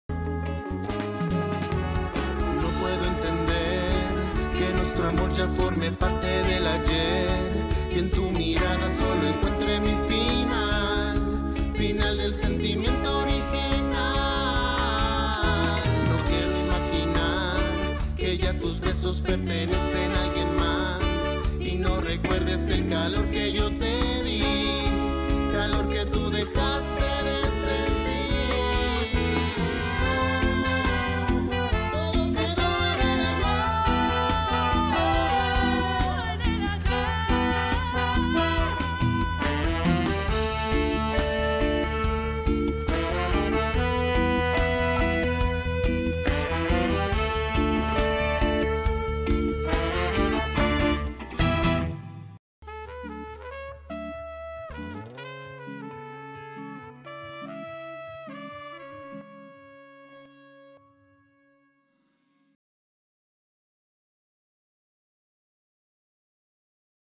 Conciertos en Faro de Oriente.
El sábado, 10 de septiembre tendremos la primera edición de varios conciertos para cerrar este 2022, año en que los shows volvieron a ver la luz en Faro de Oriente. 🤩 🥰